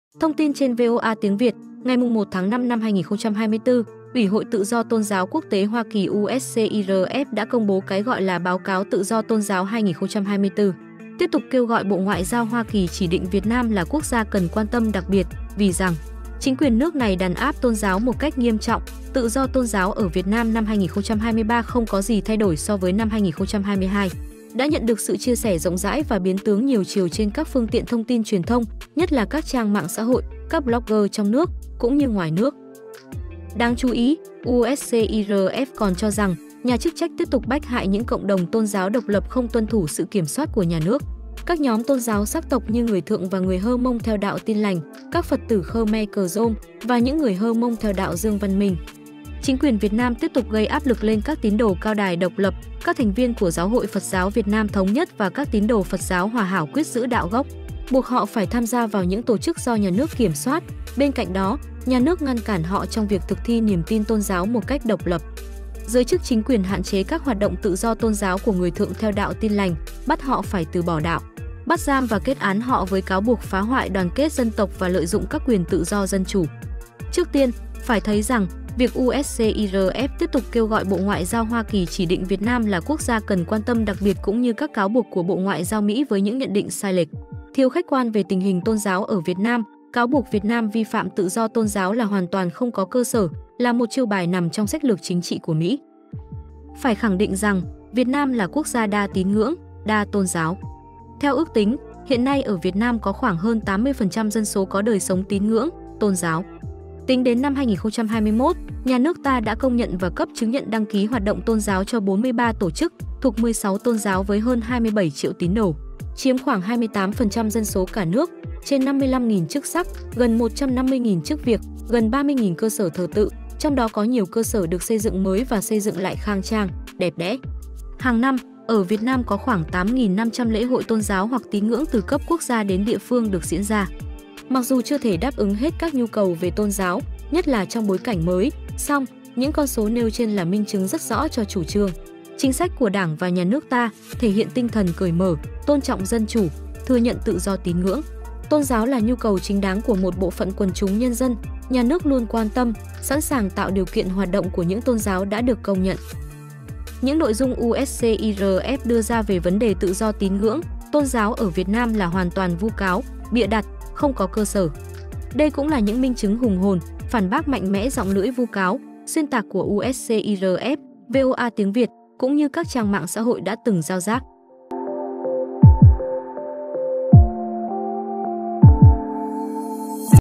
Việt Nam – Đất nước của tự do tín ngưỡng, tôn giáo!
Thông tin trên VOA Tiếng Việt, ngày 1/5/2024, Ủy hội Tự do Tôn giáo Quốc tế Hoa Kỳ (USCIRF) đã công bố cái gọi là “Báo cáo tự do tôn giáo 2024”, tiếp tục kêu gọi Bộ Ngoại giao Hoa Kỳ chỉ định Việt Nam là Quốc gia Cần quan tâm Đặc Biệt (CPC), vì rằng, chính quyền nước này “đàn áp tôn giáo một cách nghiêm trọng”; “tự do tôn giáo ở Việt Nam năm 2023 “không có gì thay đổi” so với năm 2022”!?…, đã nhận được sự chia sẻ rộng rãi và biến tướng nhiều chiều trên các phương tiện thông tin truyền thông, nhất là các trang mạng xã hội, các blogger trong nước, cũng như ngoài nước.

Đáng chú ý, USCIRF còn cho rằng, “Nhà chức trách tiếp tục bách hại những cộng đồng tôn giáo độc lập không tuân thủ sự kiểm soát của nhà nước, các nhóm tôn giáo sắc tộc như người Thượng và người Hmong theo đạo Tin Lành, các Phật tử Khmer Krom, và những người Hmong theo đạo Dương Văn Mình... Chính quyền Việt Nam tiếp tục gây áp lực lên các tín đồ Cao Đài độc lập, các thành viên của Giáo hội Phật giáo Việt Nam Thống nhất và các tín đồ Phật giáo Hoà Hảo quyết giữ đạo gốc, buộc họ phải tham gia vào những tổ chức do nhà nước kiểm soát; bên cạnh đó, nhà nước ngăn cản họ trong việc thực thi niềm tin tôn giáo một cách độc lập. Giới chức chính quyền hạn chế các hoạt động tự do tôn giáo của người Thượng theo đạo Tin Lành, bắt họ phải từ bỏ đạo, bắt giam và kết án họ với cáo buộc “phá hoại đoàn kết dân tộc và lợi dụng các quyền tự do dân chủ”!?. Hoặc “hiện vẫn có 81 nạn nhân tôn giáo theo danh sách nạn nhân tín ngưỡng (FoRB). Trong số này có 2 người vận động tự do tôn giáo cho người Khmer Krom là Danh Minh Quang và Thạch Cương…”!?

Trước tiên, phải thấy rằng, việc USCIRF tiếp tục kêu gọi Bộ Ngoại giao Hoa Kỳ chỉ định Việt Nam là Quốc gia Cần quan tâm Đặc Biệt (CPC) cũng như các cáo buộc của Bộ Ngoại giao Mỹ ngày 22/4/2024 về cái gọi là “Báo cáo nhân quyền năm 2023”, và cái gọi là “Công bố Phúc trình thường niên về tự do tôn giáo thế giới” ngày 29/4/2024 của USCIRF về tôn giáo với những nhận định sai lệch, thiếu khách quan về tình hình tôn giáo ở Việt Nam, cáo buộc Việt Nam vi phạm tự do tôn giáo là hoàn toàn không có cơ sở, là một chiêu bài nằm trong sách lược chính trị của Mỹ. Một số luận điệu vu cáo xuyên tạc rằng, các chính sách tôn giáo của Việt Nam không phù hợp với luật pháp quốc tế là vô căn cứ. Việt Nam luôn tôn trọng quyền tự do tín ngưỡng, tôn giáo của người dân; nỗ lực bảo đảm cho các tôn giáo được hoạt động bình thường trong khuôn khổ pháp luật. Việt Nam đã và đang cam kết, bảo vệ, để mọi công dân thực hiện quyền tự do tôn giáo của mình theo hiến pháp và luật pháp. Các tín đồ, các tôn giáo được tự do thực hiện nghi lễ và hoạt động tôn giáo một cách không bị ràng buộc.